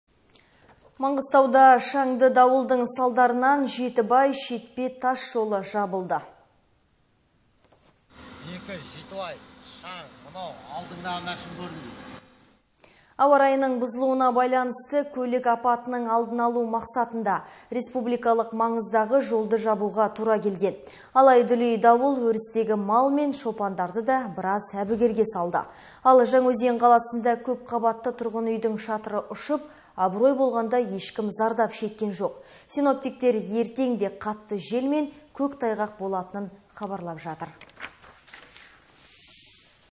Маңғыстау: Жетібай-Шетпе тасжолы жабылды
28 қаңтар 2021

Маңғыстауда шаңды дауылдың салдарынан Жетібай-Шетпе тасжолы жабылды.

Бекі-Жетібай мынау шаң алдыңдағы машина көрінбейді. Ауа-райының бұзылуына байланысты көлік апатының алдын алу мақсатында, республикалық маңыздағы жолды жабуға тура келген. Алай-дүлей дауыл өрістегі мал мен шопандарды да біраз әбігерге салды. Ал, Жаңаөзен қаласында көп қабатты тұрғын үйдің шатыры ұшып, абырой болғанда ешкім зардап шеккен жоқ. Синоптиктер ертең де қатты жел мен көктайғақ болатынын хабарлап жатыр.